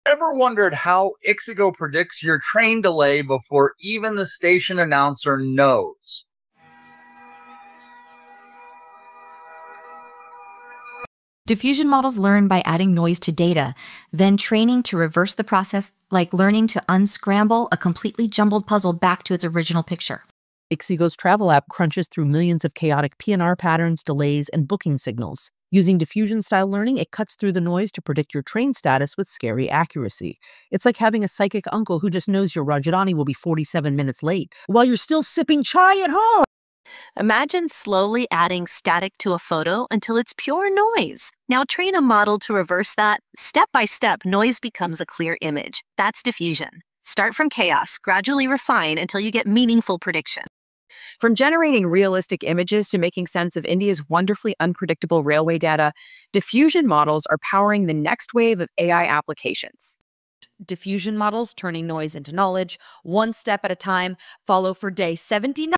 Diffusion Models Basics
How does ixigo know your train is late before you do? Diffusion models turn chaos into predictions. Mind = blown.

Your train is late. ixigo already knew. Here's the AI secret behind it.

Diffusion models work like magic - they add noise to data, then learn to reverse it. ixigo uses this to predict delays from millions of chaotic railway patterns. It's like having a psychic uncle who knows your Rajdhani will be 47 minutes late while you're still sipping chai!

Drop a "CHAOS" if you want more AI breakdowns!